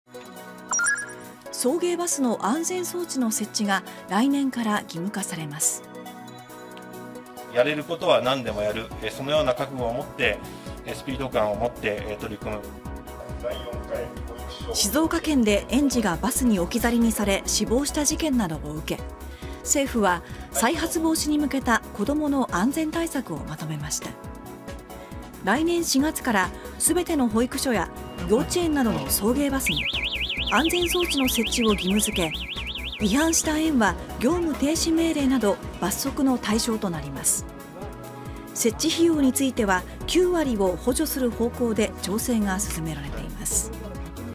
「送迎バス」の安全装置を義務化 来年4月から “9割補助”で調整(2022年10月12日)
送迎バスの安全装置の設置が、来年から義務化されます。

　小倉少子化担当大臣：「やれることはなんでもやる。そのような覚悟を持って、スピード感を持って取り組む」

　静岡県で、園児がバスに置き去りにされ死亡した事件などを受け、政府は、再発防止に向けた子どもの安全対策をまとめました。

　来年4月から、すべての保育所や幼稚園などの送迎バスに安全装置の設置を義務付け、違反した園は業務停止命令など罰則の対象となります。

　設置費用については、9割を補助する方向で調整が進められています。